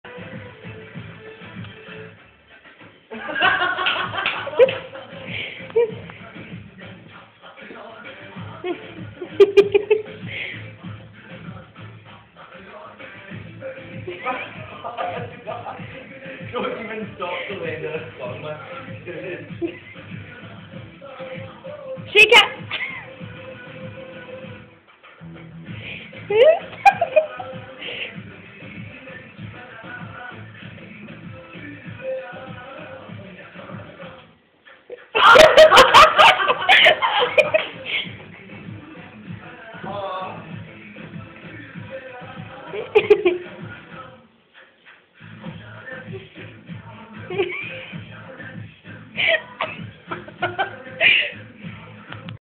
rafferz belly dancing.mp4
rafferz with my belly dancing skirt on givin it all hes got.